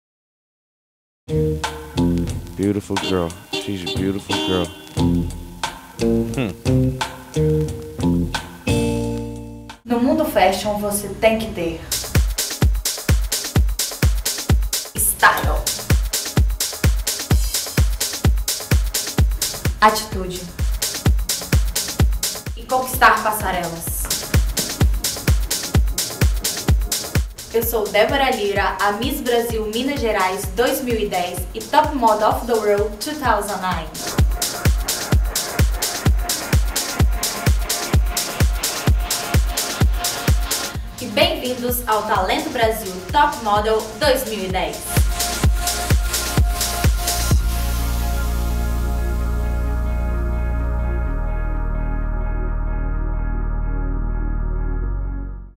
Débora Lyra - Miss Brasil 2010
Debora Lyra, Miss Minas Gerais e Miss Brasil 2010, apresenta o Talento Brasil 2009/2010 em Miami Beach. Veja o vídeo realizado na Flórida: Imagens e Fotos: JV Galindo - Edição: Neusa Martinez - Produção: Caio Castro - Maquiagem: Ronaldo Lisboa - Cabelo: JV Galindo - Roteiro: Caio Castro e JV Galindo - Realização: BRTVonline.com